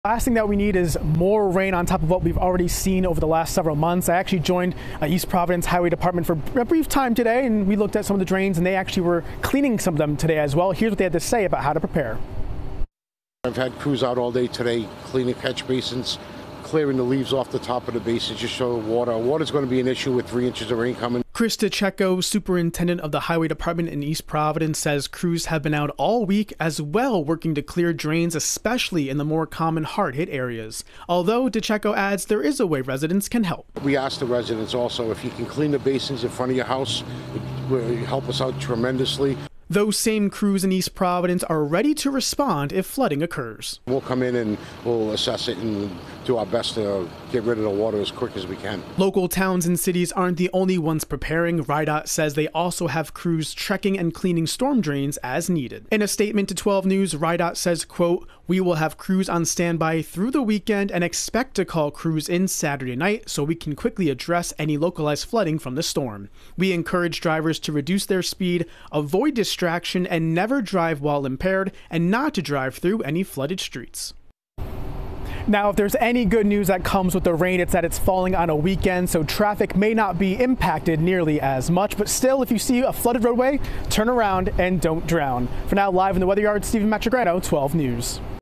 Crews preparing for this weekend's heavy rainfall
Steven Matregrano reports.